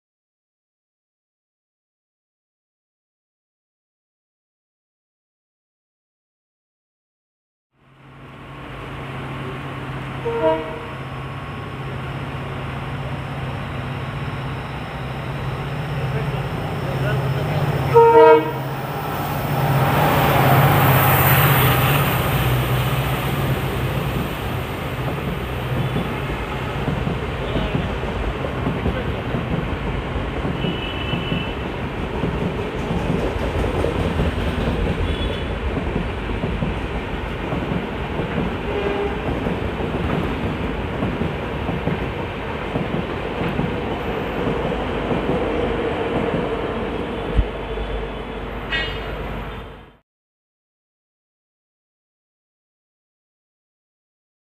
Train In Rain at Malleswaram | Indian Railways
Hello Everyone
Capturing Panchaganga Express Bound to Karwar Passes Malleswaram during Rain
Which Train easily Tackles During lightning Rain
Train In the Video
1. 16535 Ksr Bengaluru City  - Karwar Panchaganga Express with Unknown Wdp4d could be from Krishnarajapuram